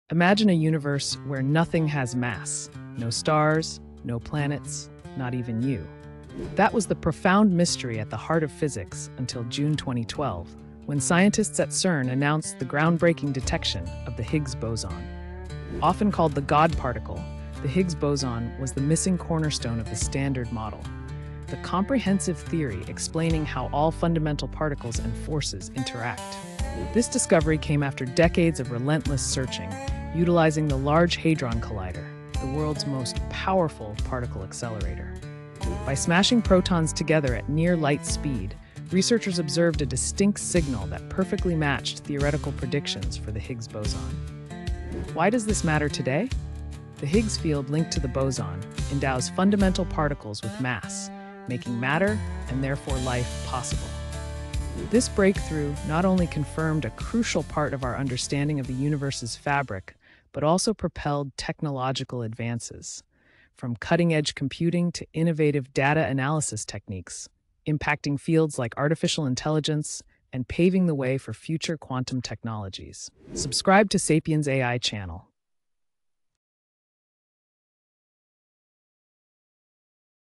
How the Higgs Boson Changed Physics Forever
In June 2012, CERN announced the discovery of the Higgs boson, revolutionizing our understanding of the universe. Find out why this matters today!